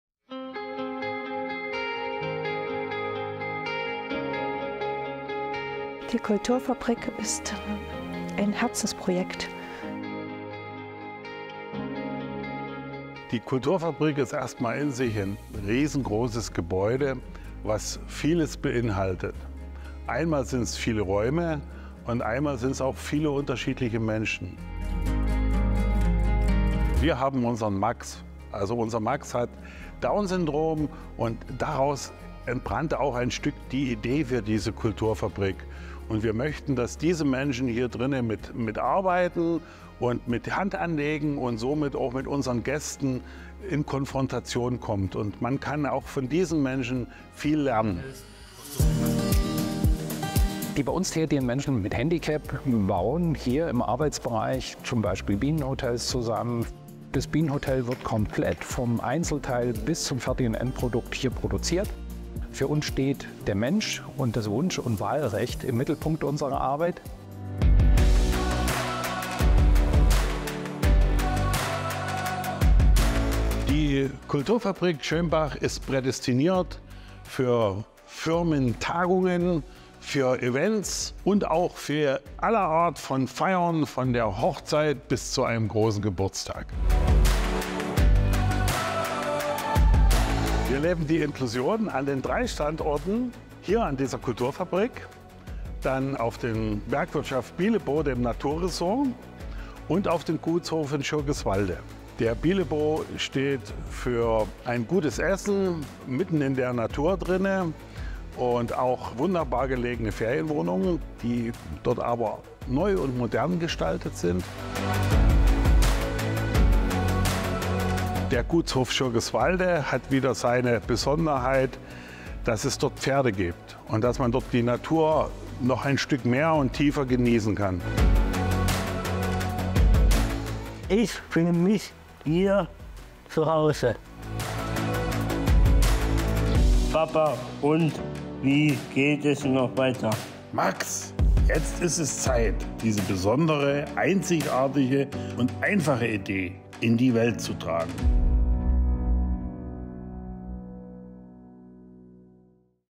DorfWaldBach | inklusive Orte für Veranstaltungen, Tagungen & Ferienwohnungen
BEIERSDORF | SCHIRGISWALDE | SCHÖNBACH
Drei Orte, drei Vorhaben, drei Projekte unter einem Dach, unter einer Familie. Ein visionäres Vorhaben, das Inklusion lebt und den Charme des Dörflichen mit innovativen Ideen wieder aufblühen lässt.

Veranstaltungen in einzigartiger Atmosphäre
Sie suchen nach dem idealen Ort für Ihre nächste Veranstaltung? Unsere Eventlocation bietet vielseitige Räume für Tagungen, Konferenzen und Feierlichkeiten. Ob Business-Event oder private Feier – bei uns sind Sie richtig! Unsere erfahrenen Eventplaner unterstützen Sie bei der Organisation und sorgen für einen reibungslosen Ablauf.

Tagungen mit Stil und Komfort
Unsere Tagungsräume sind mit modernster Technik ausgestattet und bieten Platz für kleine Gruppen bis hin zu großen Konferenzen. Nutzen Sie unsere Tagungsstätte für produktive Meetings, Workshops und Seminare. Wir sind der ideale Ort für Ihre geschäftlichen Veranstaltungen.

Ferienwohnungen Oberlausitz
Entdecken Sie unsere gemütlichen Ferienwohnungen in der Oberlausitz! Egal, ob Sie einen romantischen Kurztrip oder einen Familienurlaub planen, unsere Unterkünfte bieten den perfekten Rückzugsort. Mieten Sie Ihre Traum-Ferienwohnung und genießen Sie erholsame Tage inmitten der Natur.

Wir freuen uns darauf, Sie in unseren Ferienwohnungen und bei unseren Events begrüßen zu dürfen!